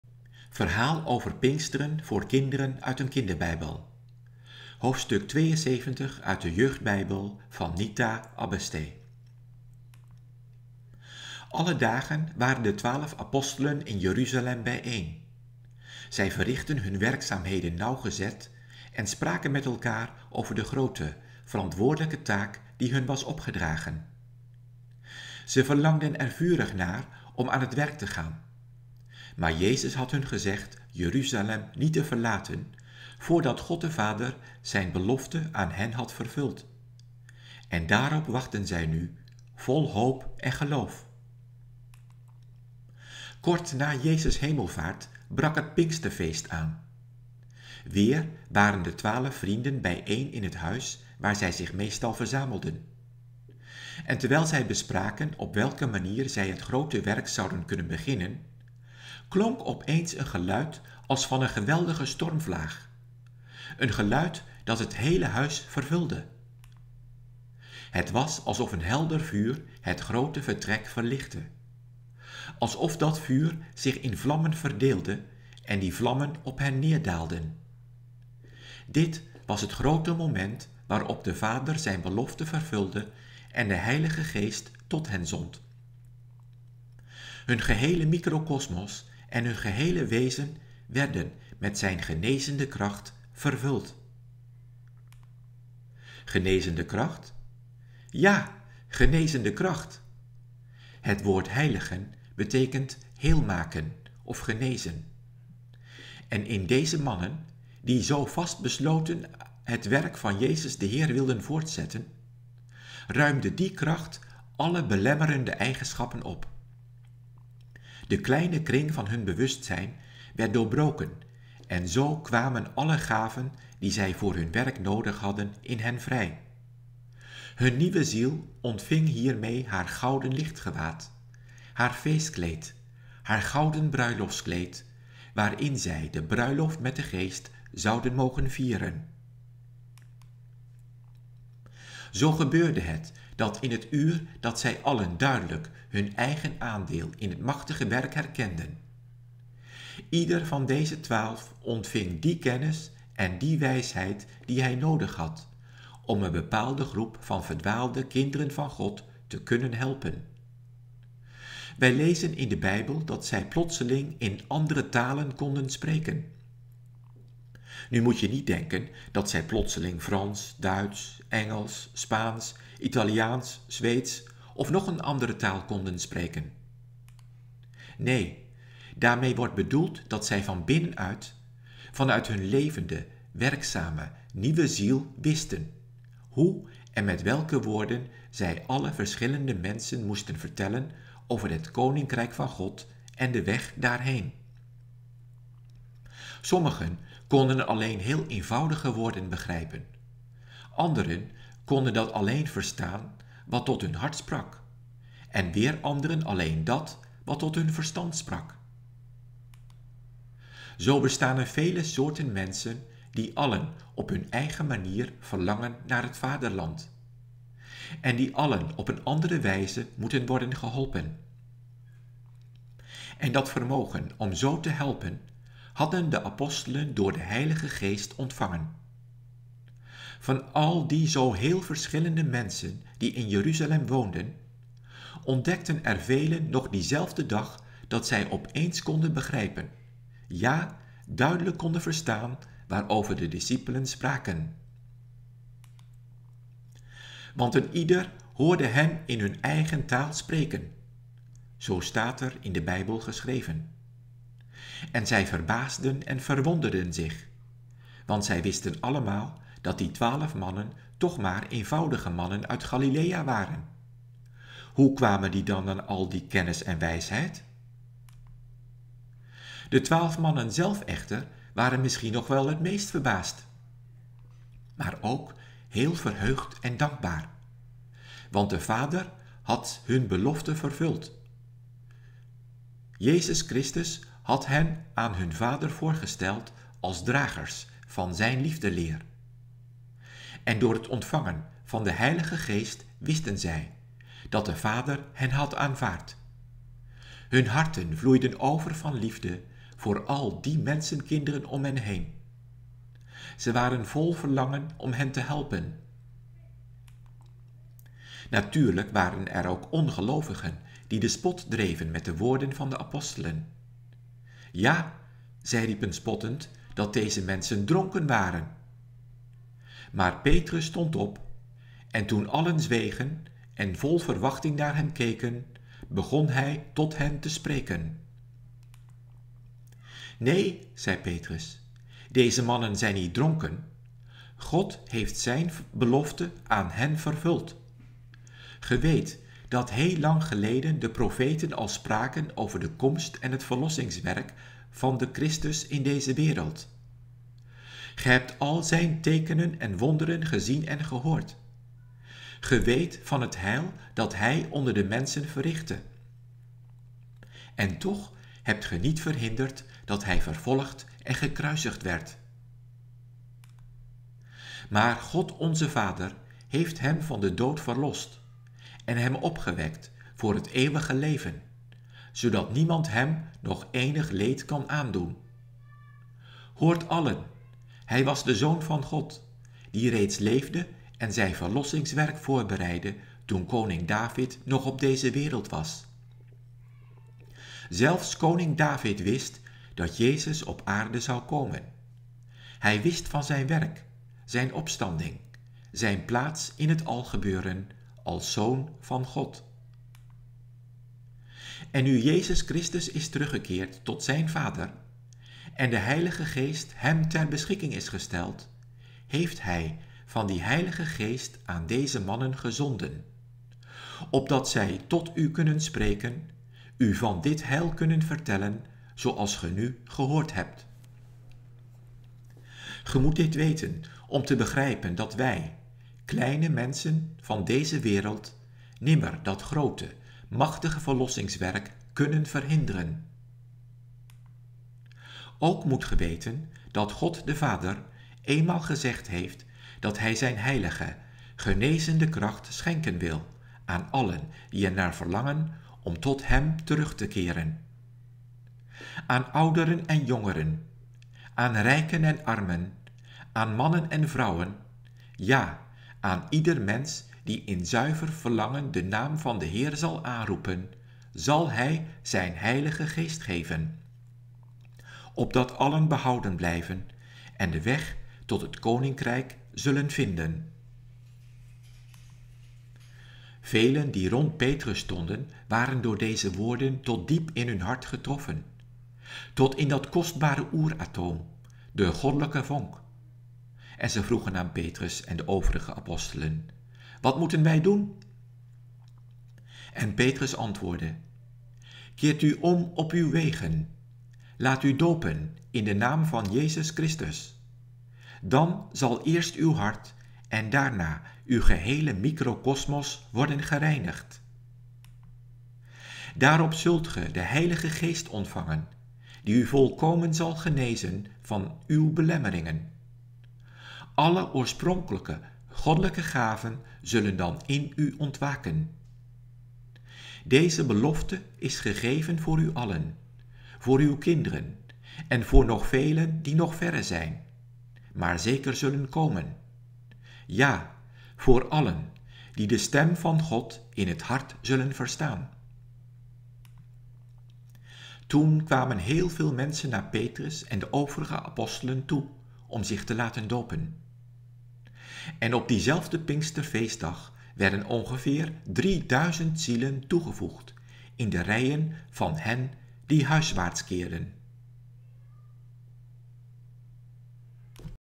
Verhaal over Pinksteren en de Heilige Geest voor kinderen uit kinderbijbel van Nita Abbestee
Alle dagen waren de twaalf apostelen in Jeruzalem bijeen. Zij verrichtten hun werkzaamheden nauwgezet en spraken met elkaar over de grote, verantwoordelijke taak die hun was opgedragen. Zij verlangden er vurig naar om aan het werk te gaan. Maar Jezus had hun gezegd Jeruzalem niet te verlaten, voordat God de Vader zijn belofte aan hen had vervuld. En daarop wachtten zij nu, vol hoop en geloof!

Kort na Jezus' hemelvaart brak het Pinksterfeest aan. Weer waren de twaalf vrienden bijeen in het huis waar zij zich meestal verzamelden. En terwijl zij bespraken op welke manier zij het grote werk zouden kunnen beginnen, klonk opeens een geluid als van een geweldige stormvlaag; een geluid dat het hele huis vervulde.

Het was alsof een helder vuur het grote vertrek verlichtte; alsof dat vuur zich in vlammen verdeelde en die vlammen op hen neerdaalden. Dit was het moment waarop de Vader zijn belofte vervulde en de Heilige Geest tot hen zond! Hun gehele microkosmos en hun gehele wezen werden met zijn genezende kracht vervuld!

Genezende kracht? Ja,  genezende kracht! Het woord 'heiligen' betekent 'heel maken' of 'genezen'. En in deze mannen, die zo vastbesloten het werk van Jezus de Heer wilden voortzetten, ruimde die kracht alle belemmerende eigenschappen op; de kleine kring van hun bewustzijn werd doorbroken en zo kwamen alle gaven die zij voor hun werk nodig hadden in hen vrij. Hun nieuwe ziel ontving hiermee haar gouden lichtgewaad, haar feestkleed, haar gouden bruiloftskleed, waarin zij de bruiloft met de Geest zouden mogen vieren.

Zo gebeurde het dat in het uur dat zij allen duidelijk hun eigen aandeel in het machtige werk herkenden. Ieder van deze twaalf ontving die kennis en die wijsheid, die hij nodig had  om een bepaalde groep van de verdwaalde kinderen van God te kunnen helpen. Wij lezen in de Bijbel dat zij plotseling in andere talen konden spreken. Nu moet je niet denken dat zij plotseling Frans, Duits, Engels, Spaans, Italiaans, Zweeds of nog een andere taal konden spreken.

Nee, daarmee wordt bedoeld dat zij van binnenuit, vanuit hun levende, werkzame nieuwe ziel wisten, hoe en met welke woorden zij alle verschillende mensen moesten vertellen over het koninkrijk van God en de weg daarheen. Sommigen konden alleen heel eenvoudige woorden begrijpen; anderen konden alleen dat verstaan wat tot hun hart sprak; en weer anderen alleen dat, wat tot hun verstand sprak.

Zo bestaan er vele soorten mensen die allen op hun eigen manier verlangen naar het Vaderland, en die allen op een andere wijze moeten worden geholpen. En dat vermogen om zo te helpen, hadden de apostelen door de Heilige Geest ontvangen! Van al die zo heel verschillende mensen die in Jeruzalem woonden, ontdekten er velen nog diezelfde dag dat zij opeens konden begrijpen, ja, duidelijk konden verstaan waarover de discipelen spraken.

'Want een ieder hoorde hen in hun eigen taal spreken!' zo staat er in de Bijbel geschreven. En zij verbaasden en verwonderden zich. Want zij wisten allemaal dat die twaalf mannen toch maar eenvoudige mannen Gallilea waren. Hoe kwamen die dan aan al die kennis en wijsheid?

De twaalf mannen zelf echter waren misschien nog wel het meest verbaasd. Maar ook heel verheugd en dankbaar! Want de Vader had zijn belofte vervuld: Jezus Christus had hen aan zijn Vader voorgesteld als dragers van zijn liefdeleer. En door het ontvangen van de Heilige Geest wisten zij, dat de Vader hen had aanvaard! Hun harten vloeiden over van liefde voor al die mensenkinderen om hen heen. Zij waren vol verlangen om hen te helpen.

Natuurlijk waren er ook ongelovigen die de spot dreven met de woorden van de apostelen. Ja, zij riepen spottend, dat deze mensen dronken waren. Maar Petrus stond op, en toen allen zwegen en vol verwachting naar hem keken, begon hij tot hen te spreken.

Spirituele teksten
Pentagram boekwinkel
Bakenessergracht 1
2011 JS Haarlem